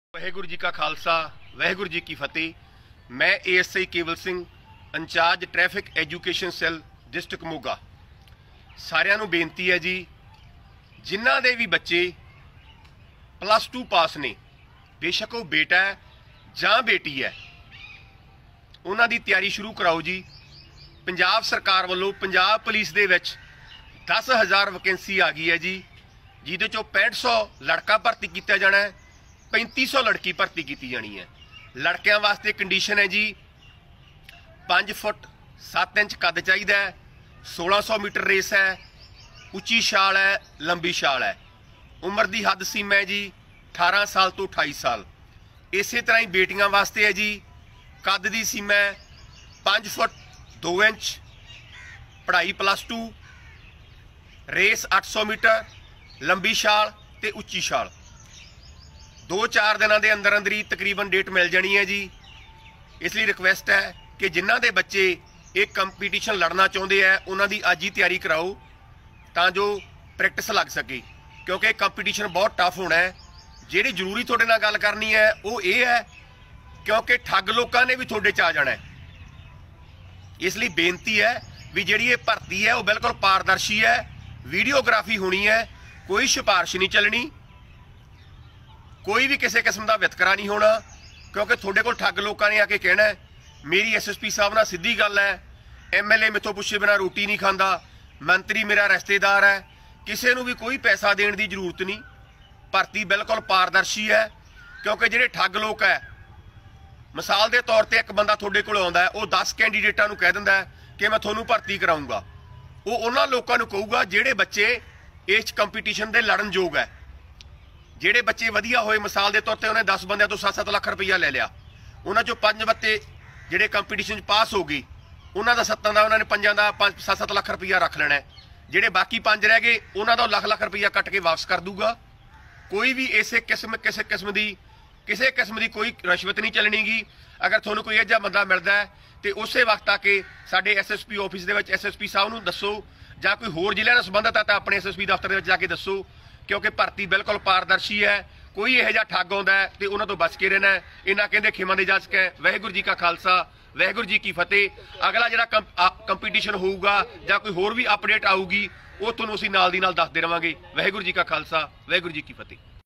ਪੰਜਾਬ ਪੁਲਿਸ ਭਰਤੀ 10 ਹਜ਼ਾਰ ਨਵੇਂ ਦਸਵੀਂ ਤੇ +2 ਪਾਸ ਬੱਚਿਆਂ ਲਈ ਸੁਨਹਿਰੀ ਮੌਕਾ | Punjab Police Jobs
Punjab Police 10000 Jobs
 Punjab Police 10000 Jobs For Boys & Girls
ਪੰਜਾਬ ਪੁਲਿਸ ਭਰਤੀ 10 ਹਜ਼ਾਰ ਨਵੇਂ ਦਸਵੀਂ ਤੇ +2 ਪਾਸ ਬੱਚਿਆਂ ਲਈ ਸੁਨਹਿਰੀ ਮੌਕਾ

And Press To Bell Icon For New Updates